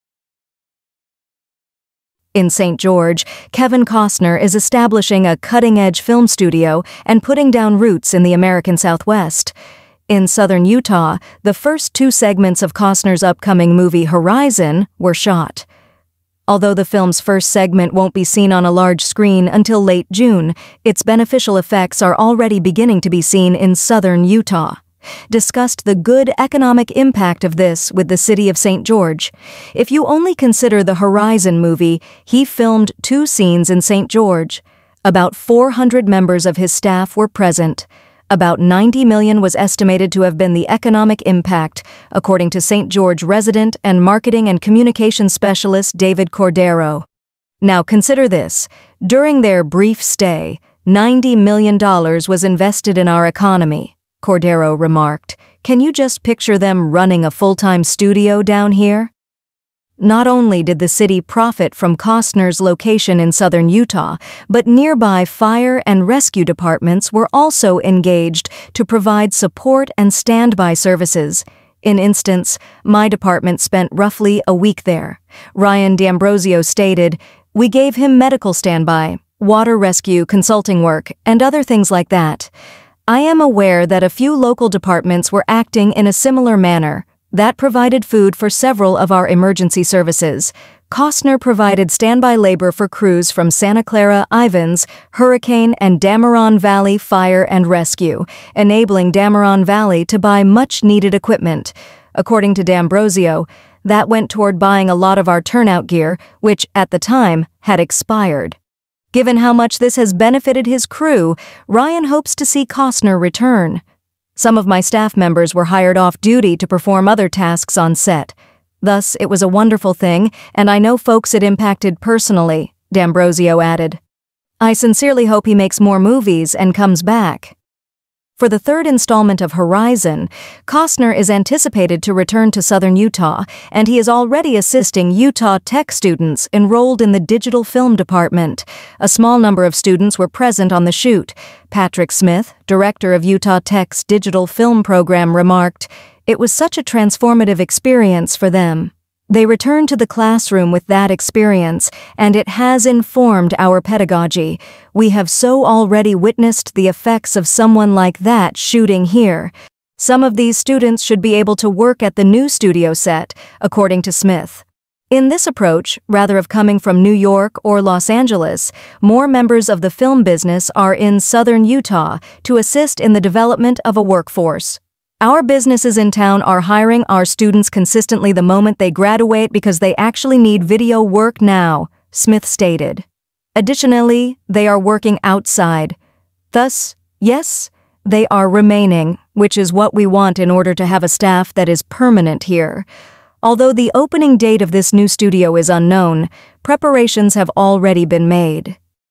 Kevin Costner brings movie magic to southern Utah
In St. George, Kevin Costner is establishing a cutting-edge film studio and putting down roots in the American Southwest.

In southern Utah, the first two segments of Costner's upcoming movie "Horizon" were shot. Although the film's first segment won't be seen on a large screen until late June, its beneficial effects are already beginning to be seen in southern Utah.

discussed the good economic impact of this with the City of St. George.

"If you only consider the 'Horizon' movie, he filmed two scenes in St. George. About 400 members of his staff were present. About $90 million was estimated to have been the economic impact, according to St. George resident and marketing and communications specialist David Cordero.

"Now consider this: during their brief stay, $90 million was invested in our economy," Cordero remarked. "Can you just picture them running a full-time studio down here?"

Not only did the city profit from Costner's location in southern Utah, but nearby fire and rescue departments were also engaged to provide support and standby services.

In instance, my department spent roughly a week there. Ryan D'Ambrosio stated, "We gave him medical standby, water rescue consulting work, and other things like that. "I am aware that a few local departments were acting in a similar manner. That provided food for several of our emergency services.

Costner provided standby labor for crews from Santa Clara-Ivins, Hurricane, and Dammeron Valley Fire and Rescue, enabling Dammeron Valley to buy much-needed equipment.

According to D'Ambrosio, "that went toward buying a lot of our turnout gear, which, at the time, had expired."

Given how much this has benefited his crew, Ryan hopes to see Costner return.

"Some of my staff members were hired off-duty to perform other tasks on set. Thus, it was a wonderful thing and I know folks it impacted personally, D'Ambrosio added. "I sincerely hope he makes more movies and comes back."

For the third installment of "Horizon," Costner is anticipated to return to southern Utah, and he is already assisting Utah Tech students enrolled in the digital film department.

A small number of students were present on the shoot. Patrick Smith, director of Utah Tech's digital film program, remarked, "It was such a transformative experience for them." They returned to the classroom with that experience, and it has informed our pedagogy. We have so already witnessed the effects of someone like that shooting here.

Some of these students should be able to work at the new studio set, according to Smith. In this approach, rather of coming from New York or Los Angeles, more members of the film business are in southern Utah to assist in the development of a workforce.

"Our businesses in town are hiring our students consistently the moment they graduate because they actually need video work now," Smith stated. Additionally, they are working outside. Thus, yes, they are remaining, which is what we want in order to have a staff that is permanent here.

Although the opening date of this new studio is unknown, preparations have already been made.